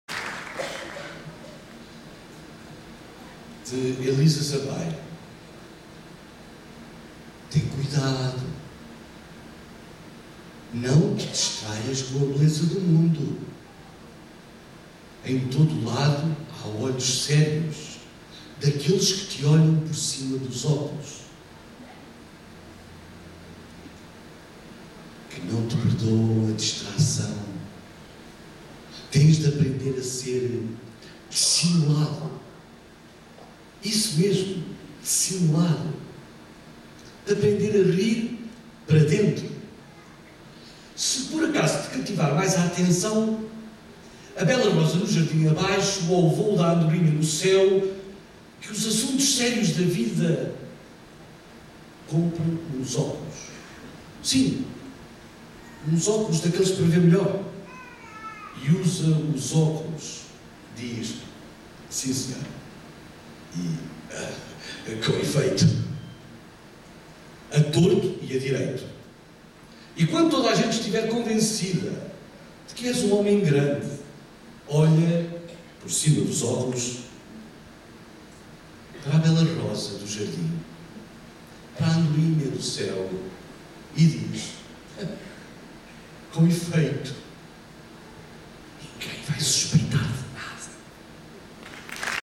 Leitura do poema "Olhos de Menino", de Elisa Sampaio, Por Carlos M. Cunha
Lançamento de "Antologia de Poesia - Entre o Sono e o Sonho Vol.V" no passado dia 22 de Março, no Salão Preto e Prata do Casino do Estoril.